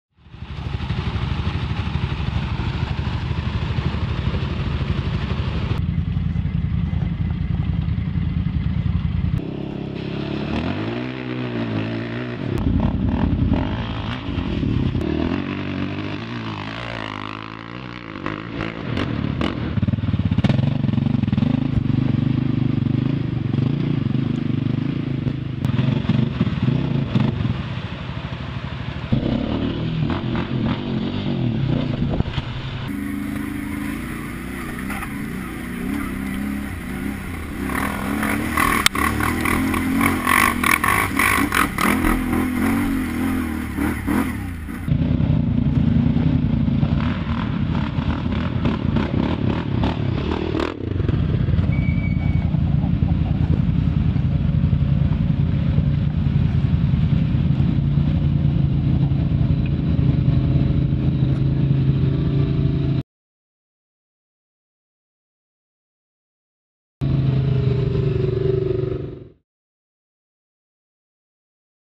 Honda 700xx _ Taking On WINDROCK Atv Park
Honda 700xx taking on the trails of Windrock Atv Park

700xx Mods:
Kenda Kutters
HMF slip-on exhaust
Honda Racing skid plates